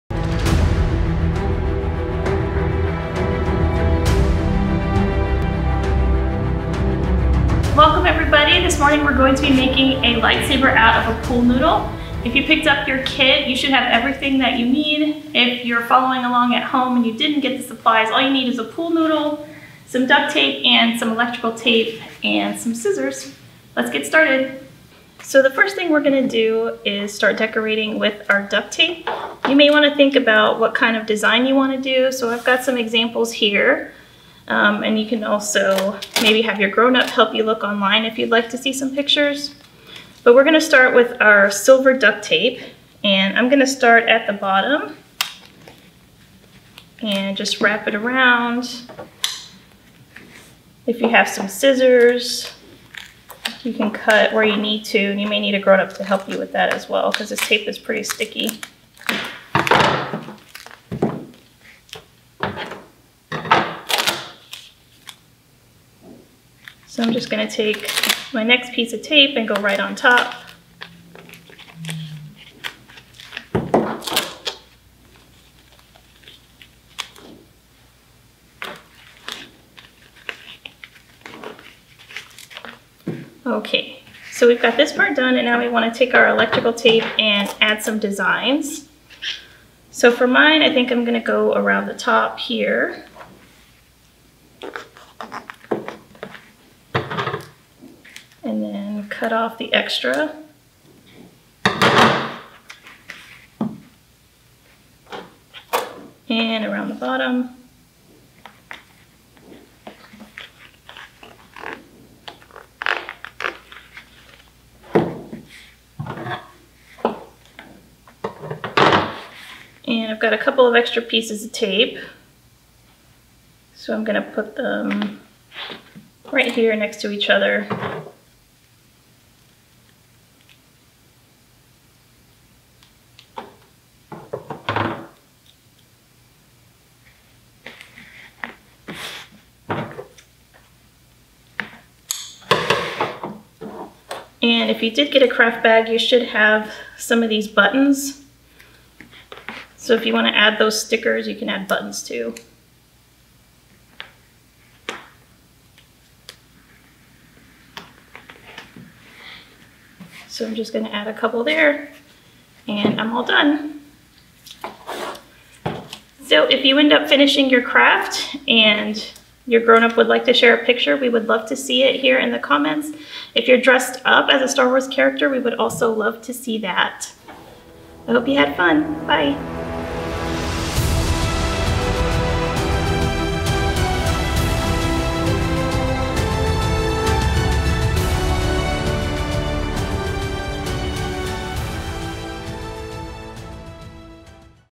2020 Library Con - Lightsaber Building Activity
Thank you for joining us for our 5th Annual Pflugerville Library Con, which is virtual this year! Learn more about this event at pflugervilletx.gov/librarycon.

This session is the Lightsaber Building Activity.